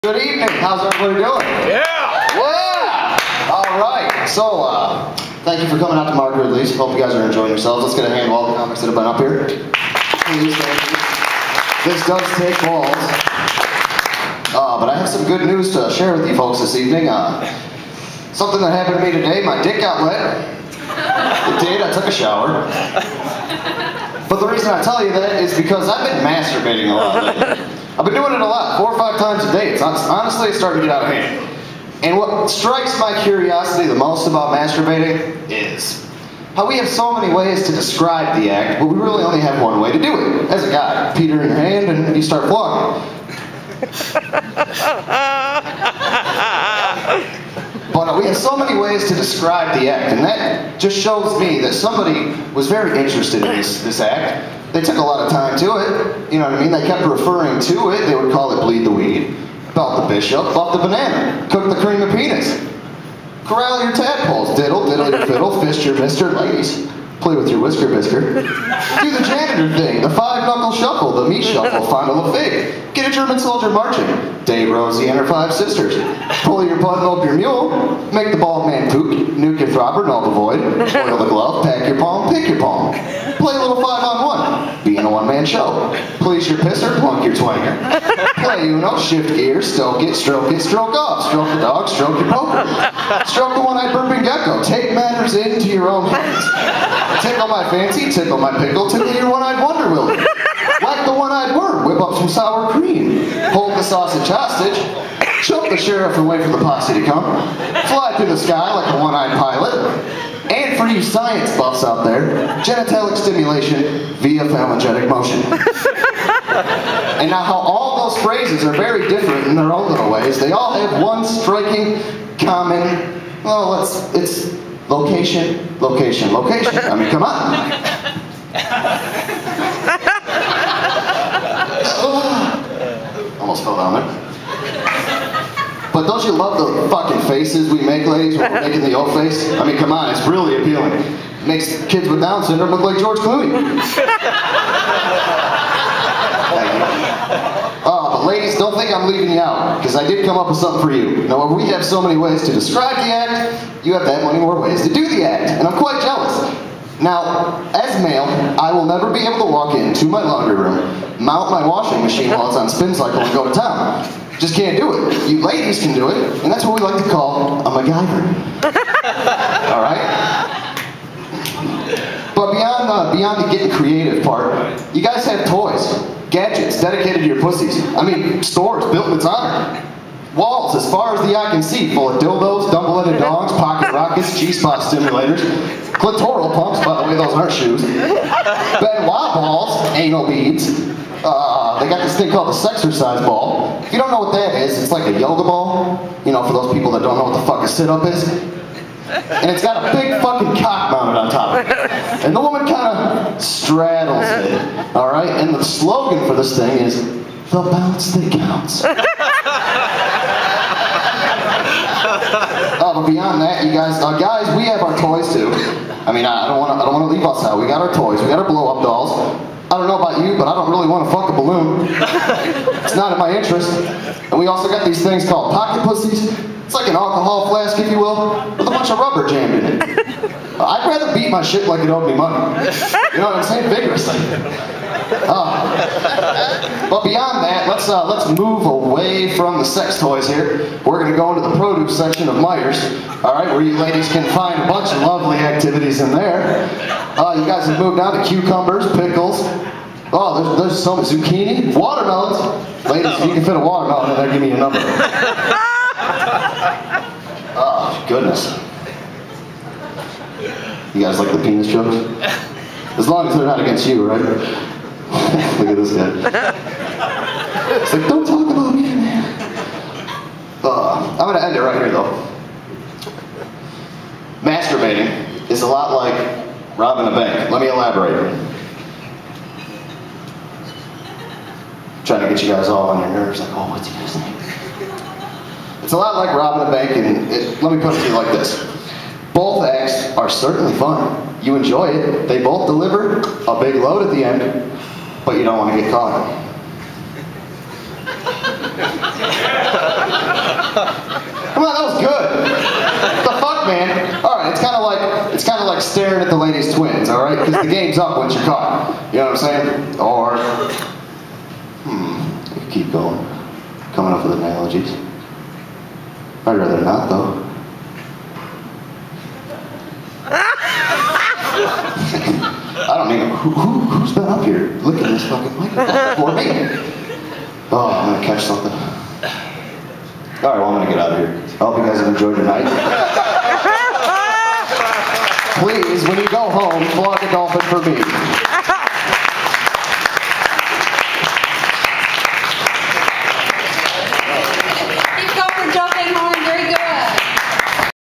Masturbation
Joe VanHorn @ Mark Ridley's Comedy Castle in Royal Oak Michigan. 5/13/09